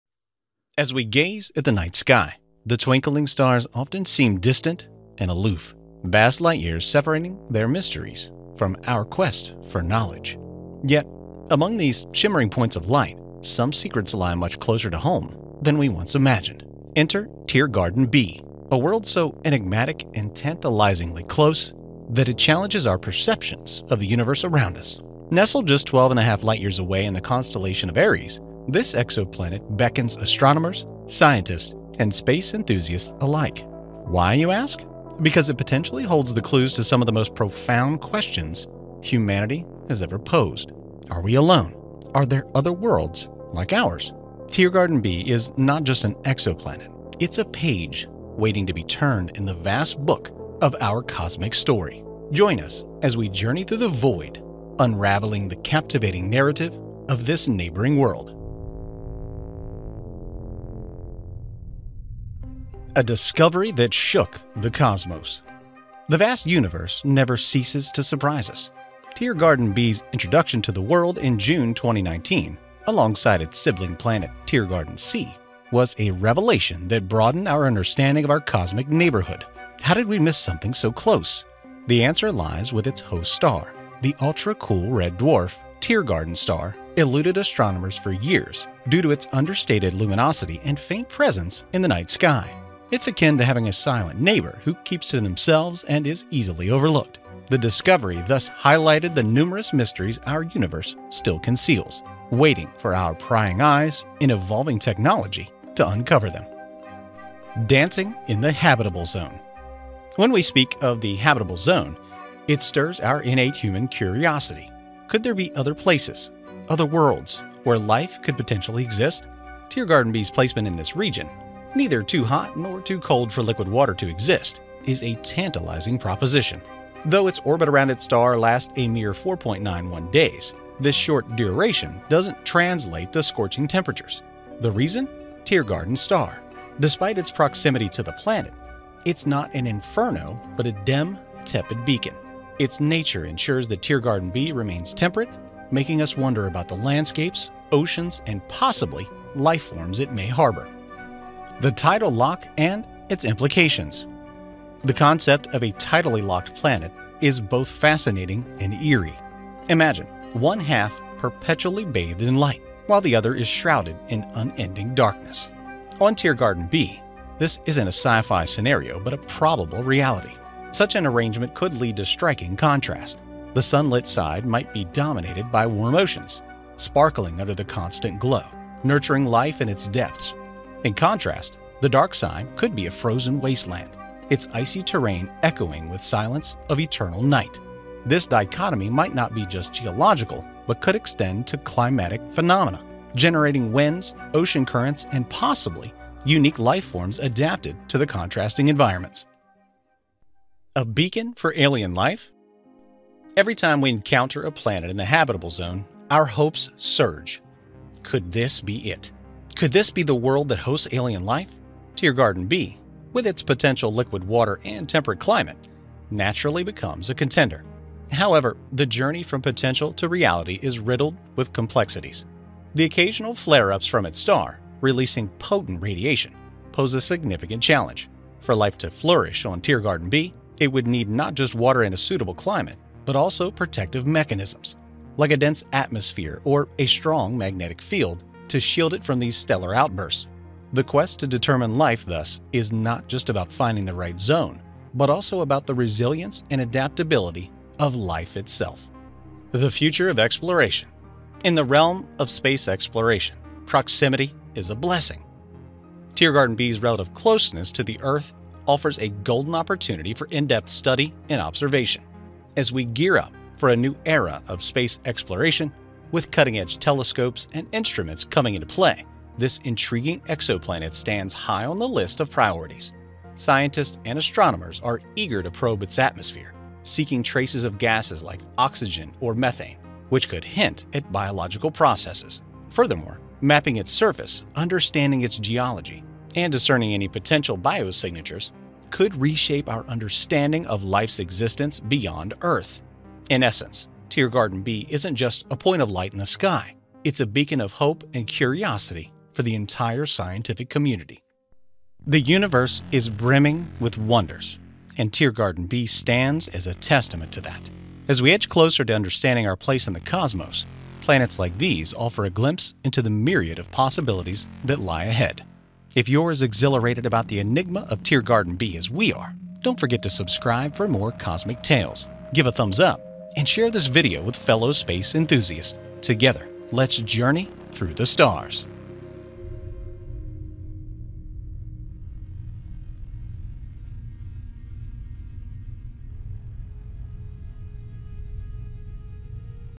We Have Discovered A Planet Even Better for Life Than Earth
Dive into the mysteries of the universe with our latest video exploring Teegarden b, an exoplanet that promises new insights into potential life beyond Earth. Nestled a mere 12.5 light-years away in the Aries constellation, Teegarden b intrigues astronomers and space enthusiasts with its habitable zone placement and potential for life.

From its discovery that left us astounded to its unique tidal lock phenomena and its promise as a beacon for alien life, this video reveals all. Don't miss out on exploring why this exoplanet might be even better suited for life than our very own Earth.

Timestamps:
0:00 Intro
1:02 A Discovery that Shook the Cosmos
1:48 Dancing in the Habitable Zone
2:31 The Tidal Lock and its Implications
3:20 A Beacon for Alien Life?
4:09 The Future of Exploration

What's you'll see:
✅ Stunning Discovery: The unveiling of Teegarden b in June 2019 expanded our cosmic horizons, introducing us to a world closer than we'd ever imagined.

✅ Life's Potential Playground: Teegarden b's position in the habitable zone, its temperate climate, and the possibilities of liquid water make it a compelling candidate for extraterrestrial life.

✅ A World of Contrasts: The probable tidal lock of Teegarden b suggests a landscape where one side always faces its star while the other remains in perpetual darkness, leading to distinct environments.

✅ In Search of Alien Life: Despite its potential for supporting life, challenges arise from the planet's proximity to its star, emphasizing the need for protective mechanisms against radiation.

✅ A New Era of Exploration: The planet's closeness to Earth makes it a prime focus for future astronomical studies, potentially rewriting our understanding of life beyond our planet.